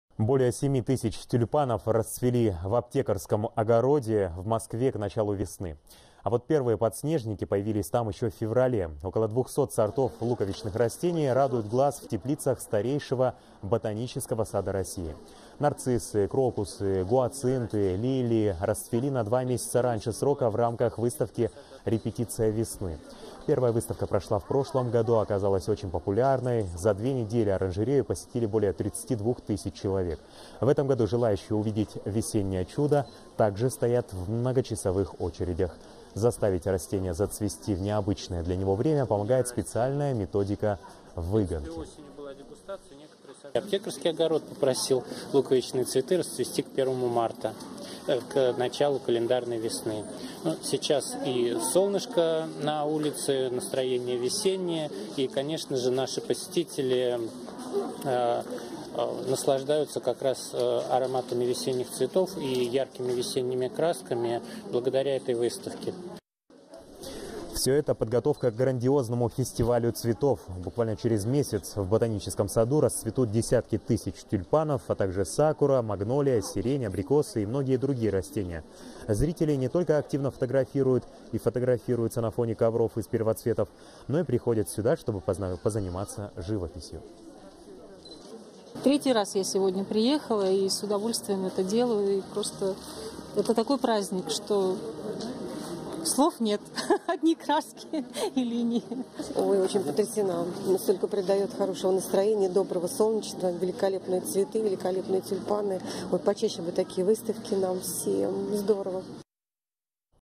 Москвичи стоят часами в очереди, чтобы увидеть «Репетицию весны»
Более 7 тысяч тюльпанов расцвели в Аптекарском огороде в Москве к началу весны. Сайт телеканала